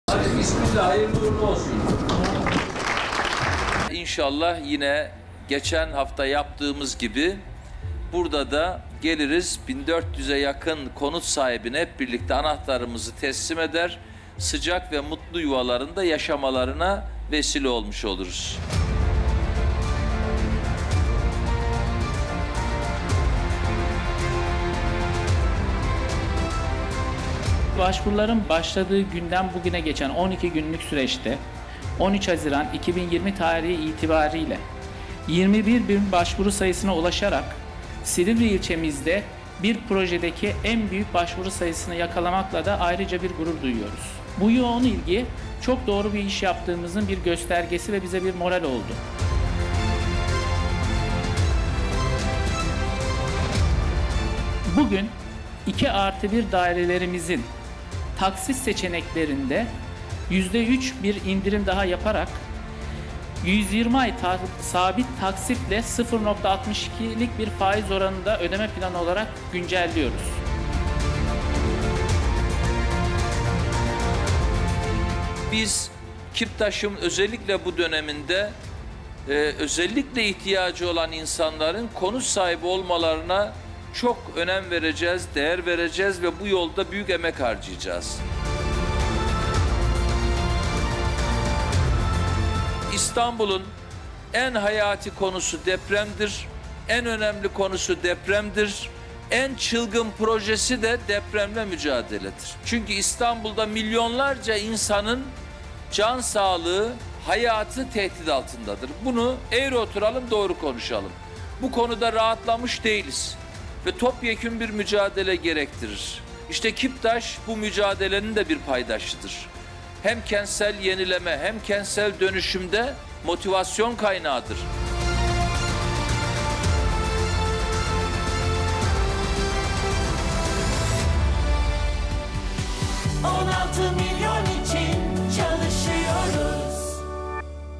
Başkan Ekrem İmamoğlu KİPTAŞ Silivri 4. Etap Sosyal Konutları'nın Temelini Attı
Başkan Ekrem İmamoğlu, dar gelirlileri ev sahibi yapacak KİPTAŞ’ın Silivri 4. Etap Konutları’nın temelini attı.

ayrıca İBBTV’yi diğer mecralarda da takip etmek için:
Makaleler, programlar ve canlı yayın: